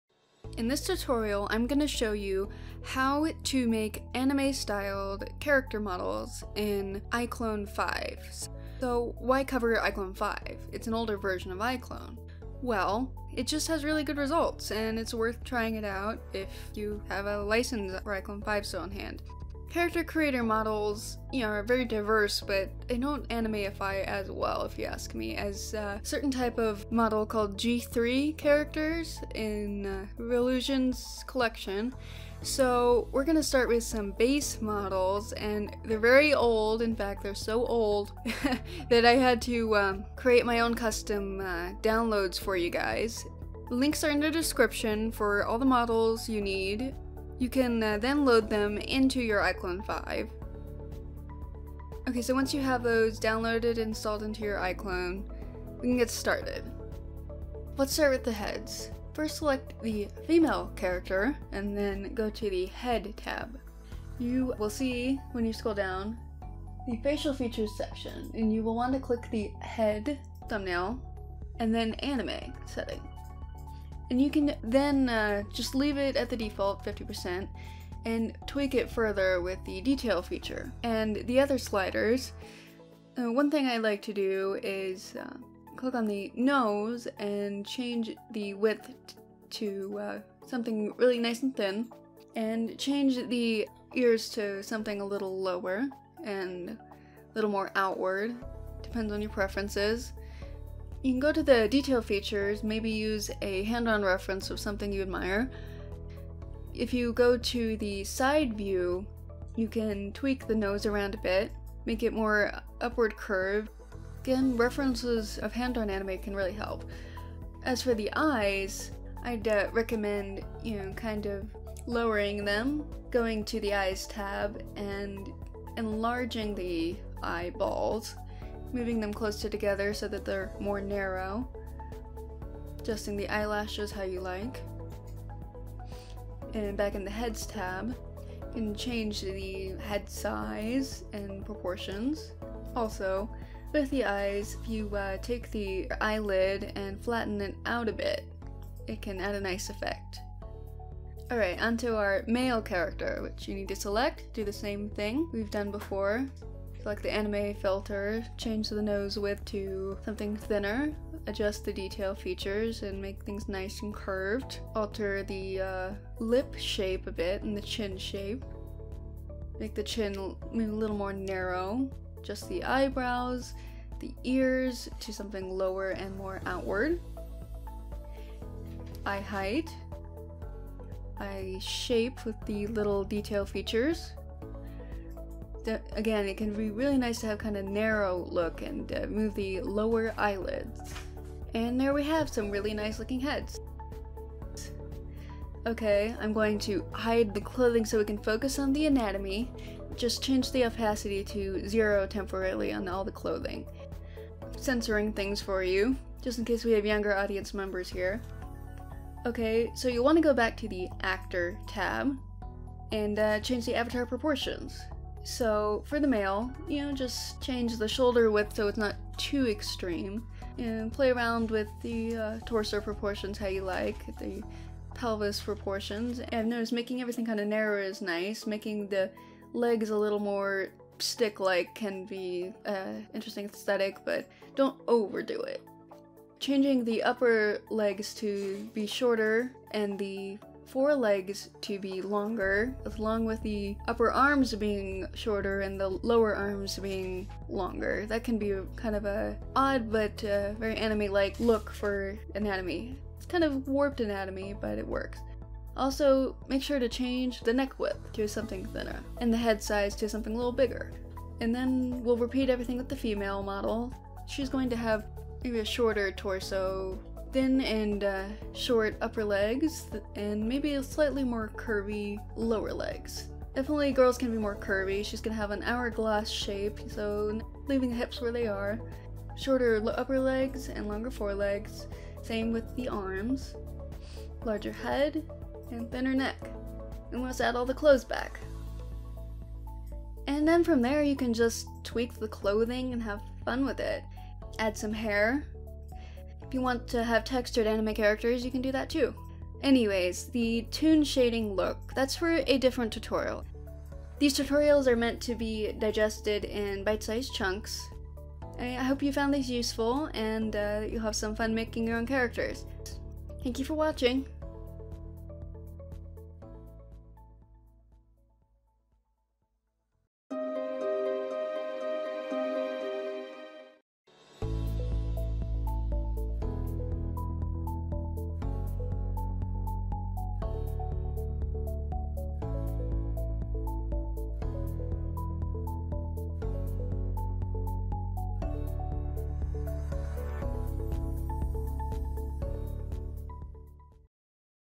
iClone Anime Tutorial #1 | Making Anime Characters in iClone 5
In this new tutorial series, I'll teach you how I use iClone to make authentic looking animes and cartoons!